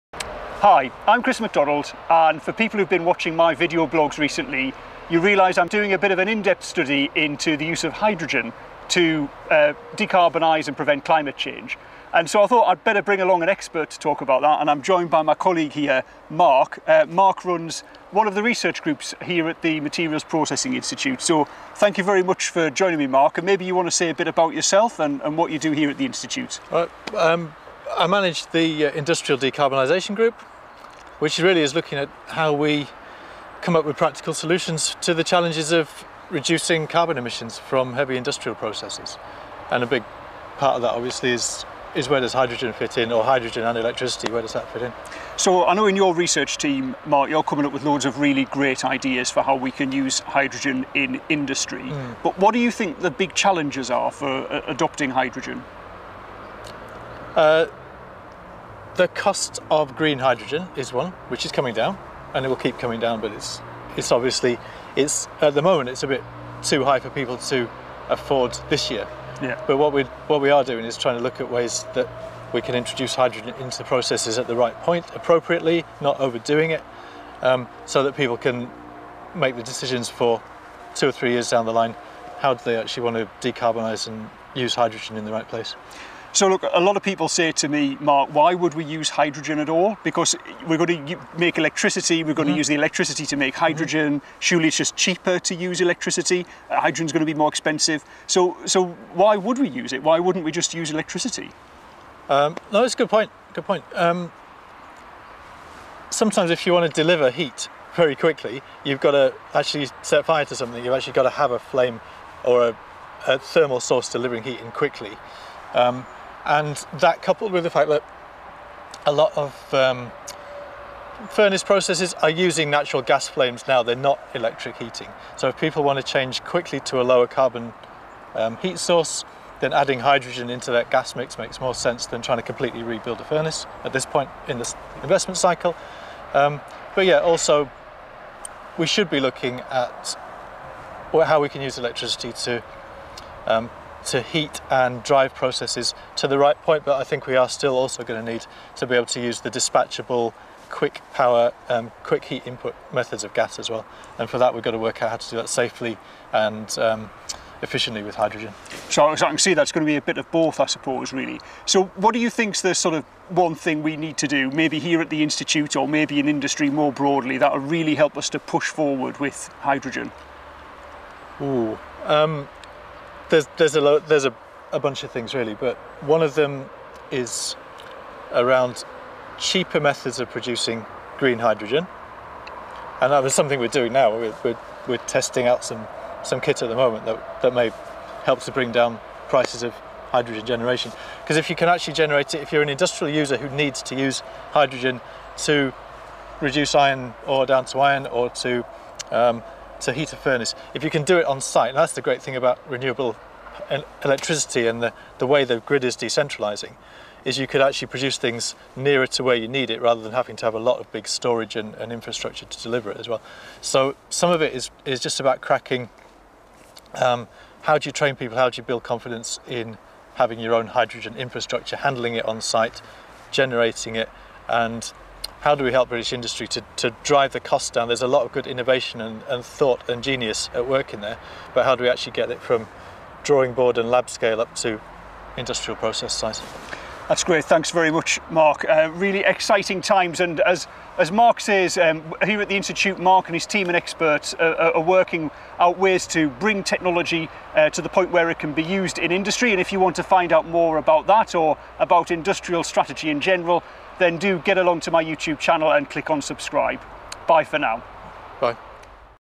Challenges for the Hydrogen Economy
Carbon emissions can be prevented by switching to hydrogen, but there are many technological and economic challenges. Here I talk to Mark Allan, who leads the Industrial Decarbonization research team at the Materials Processing Institute.